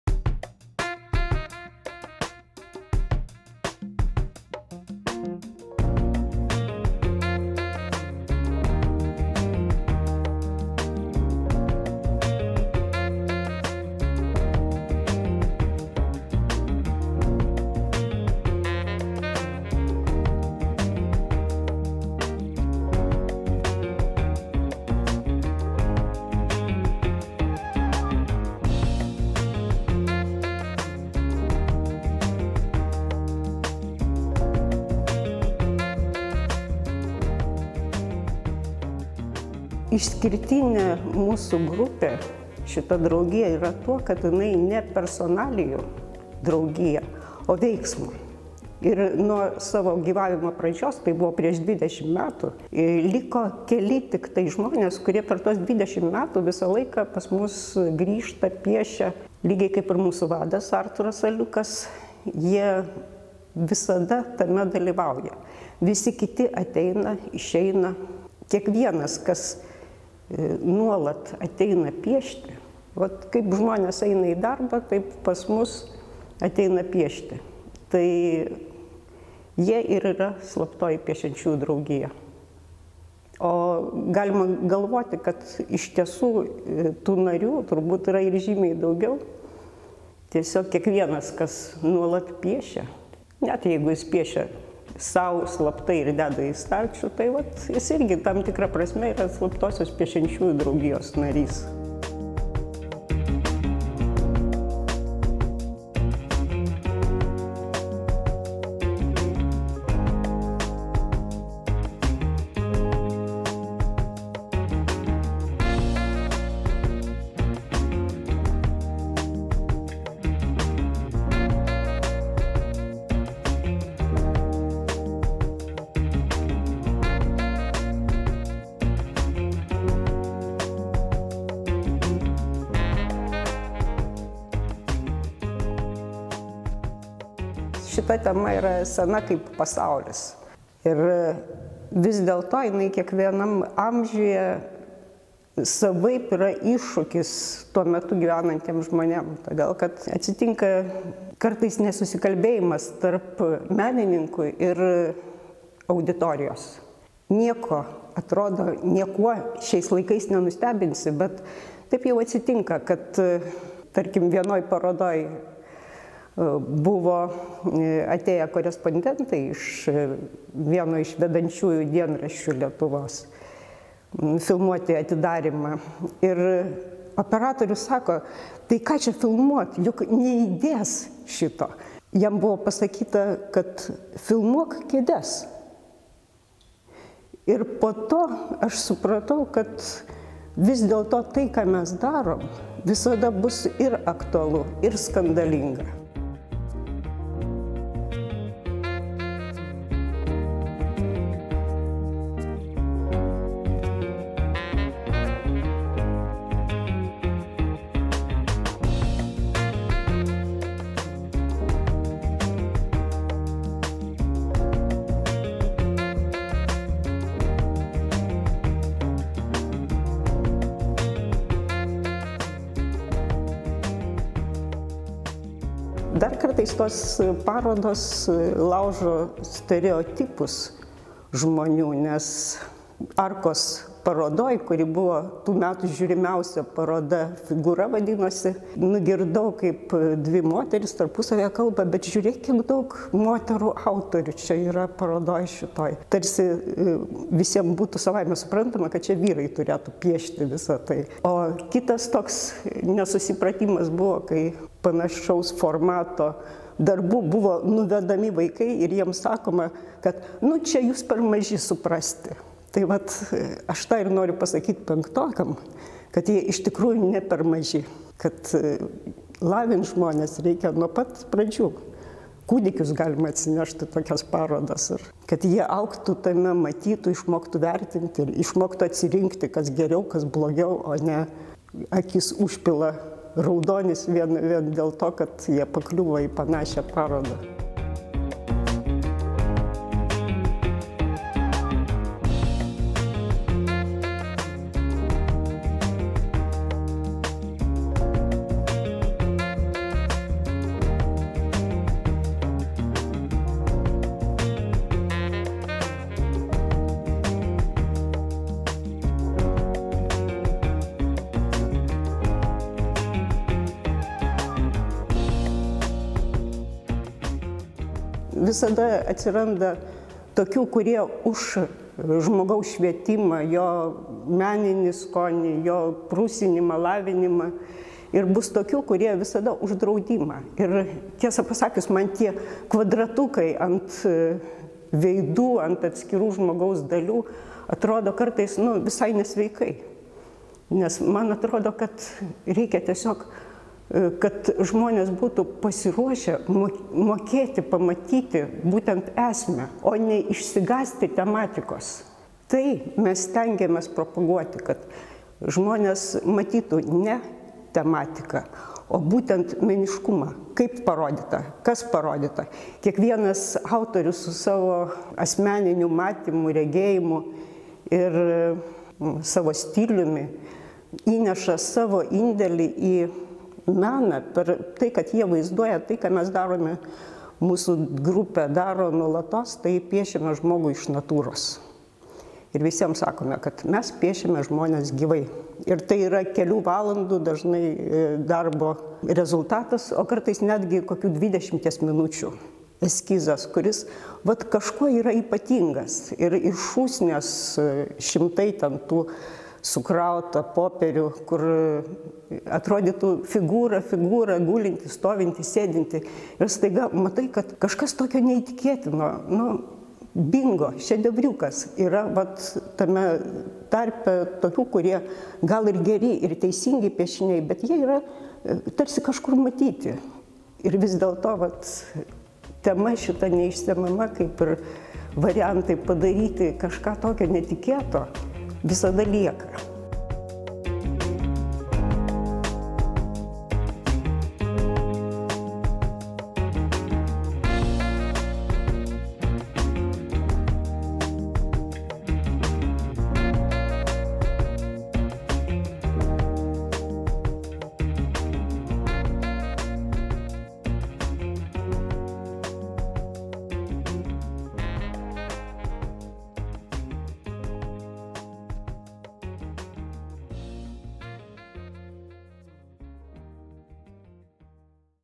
Slaptoji Piešiančiųjų Draugija | devintoji piešinių paroda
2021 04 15 – 04 30
Lietuvos dailininkų sąjungos parodų salė (Vokiečių g. 4, Vilnius).

Parodą pristatė Slaptosios Piešiančiųjų Draugijos narė Liuda Jaruševičiūtė Stankevičienė.

Videoprodukcija - Andrius Grigalaitis

2021 Lietuvos dailininkų sąjunga